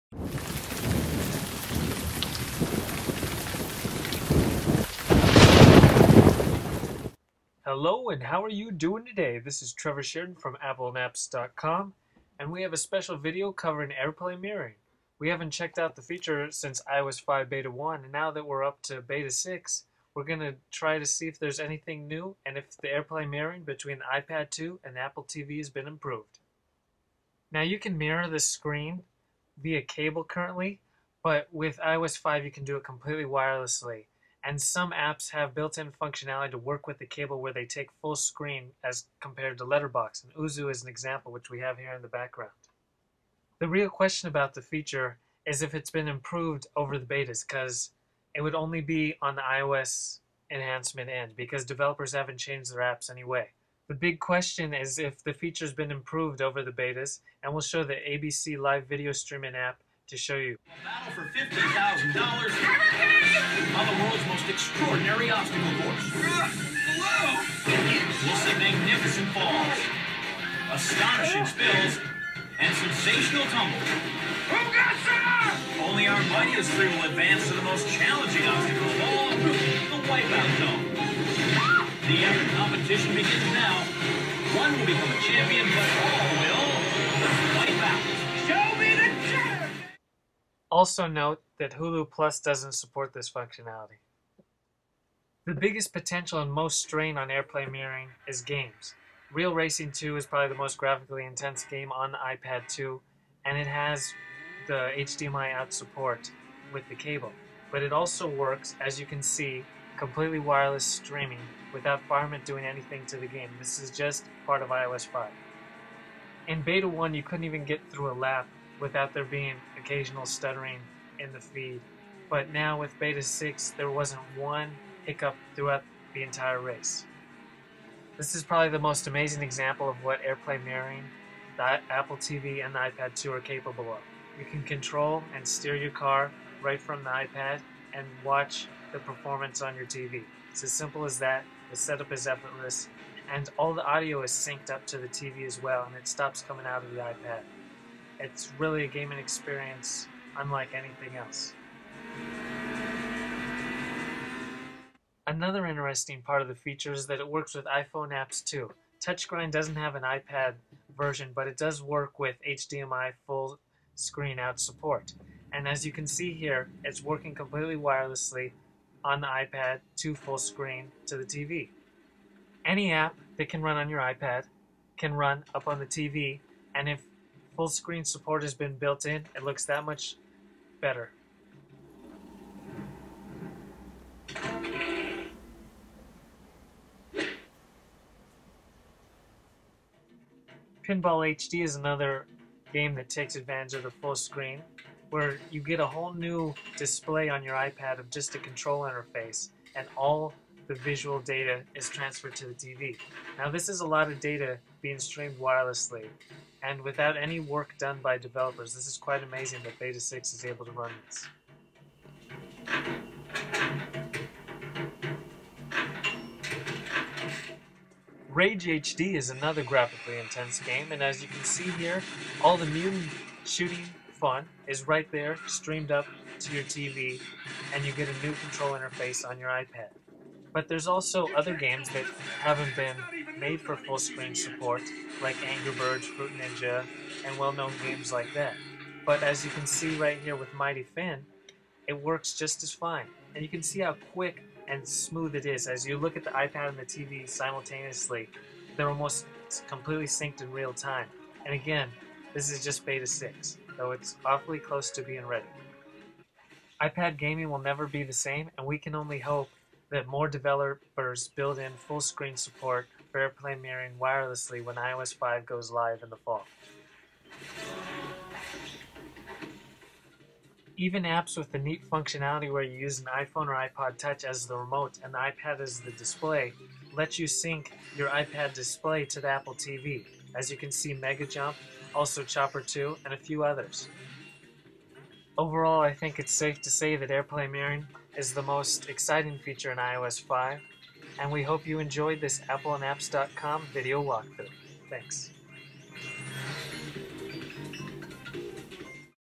AirPlay Mirroring on iPad 2 As Of iOS 5 Beta 6
Video walkthrough of AirPlay Mirroring on iPad 2 with iOS 5 Beta 6. The video attempts to show if there are any improvements since iOS 5 Beta 1.